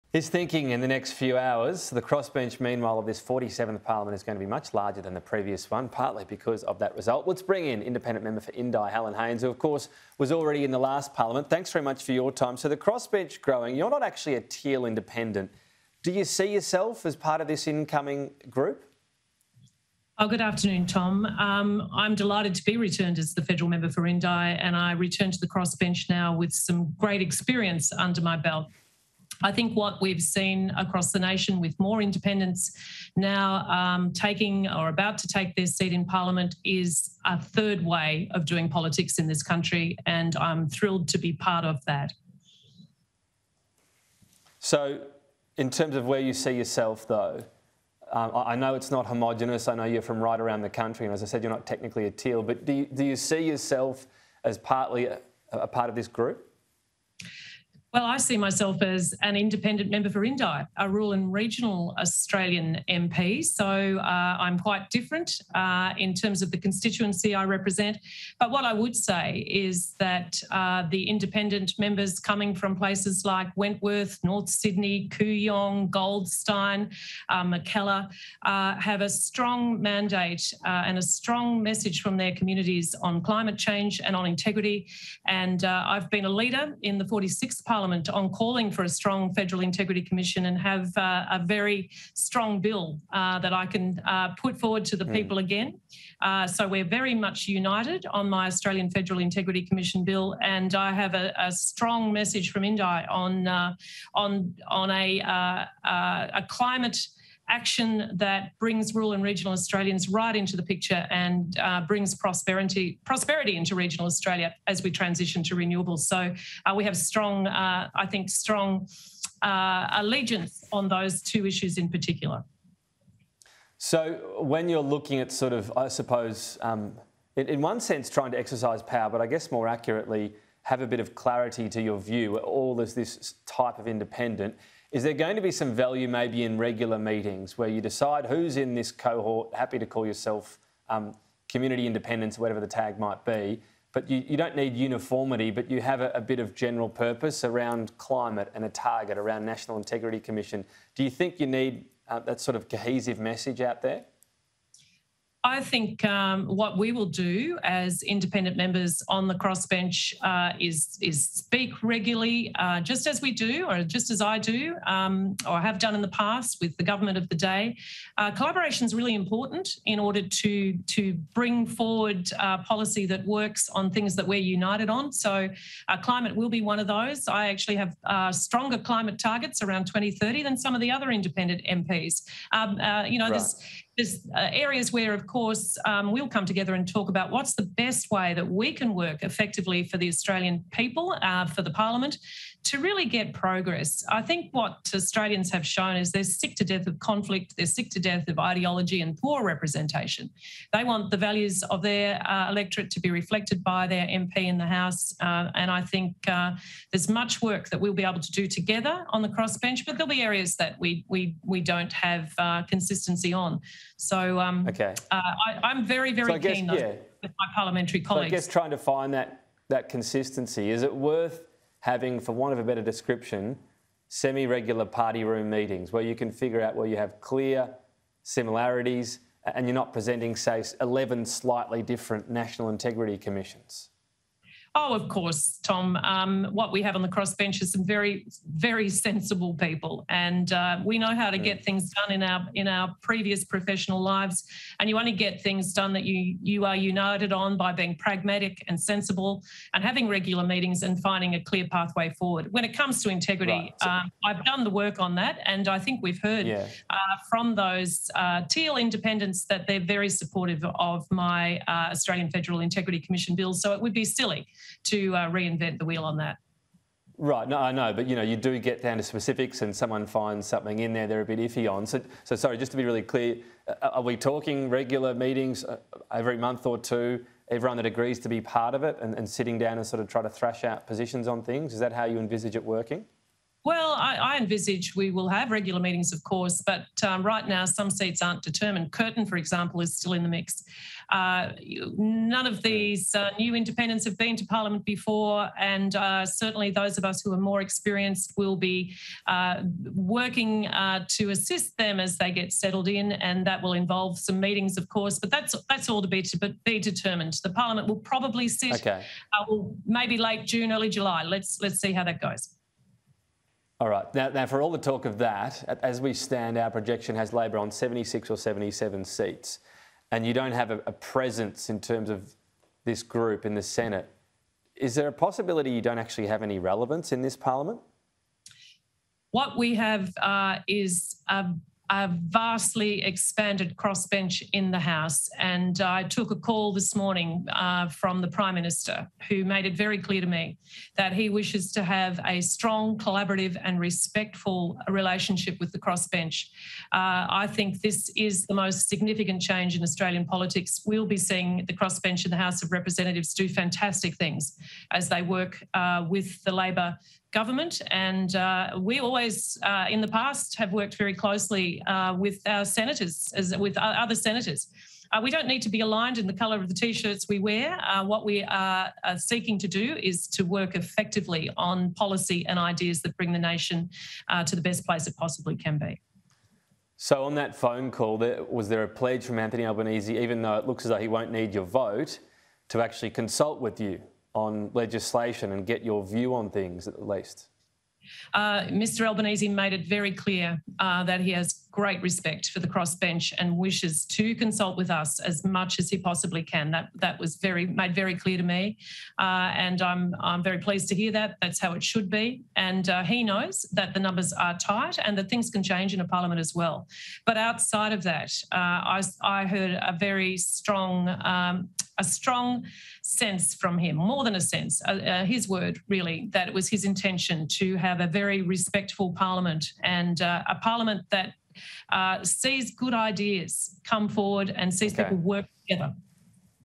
Albanese to consult with crossbench 'as much as he possibly can': Helen Haines
Independent MP Helen Haines says she took a call from Prime Minister Anthony Albanese, who made it clear that he wishes to have a “strong, collaborative, and respectful” relationship with the crossbench.

“What we have is a vastly expanded crossbench in the house,” Ms Haines told Sky News Australia.

“I think this is the most significant change in Australian politics; we’ll be seeing the crossbench in the House of Representatives do fantastic things as they work with the Labor government.

“Mr Albanese made it very clear that he has great respect for the crossbench and wishes to consult with us as much as he possibly can, that was made very clear to me, and I’m very pleased to hear that; that’s how it should be.”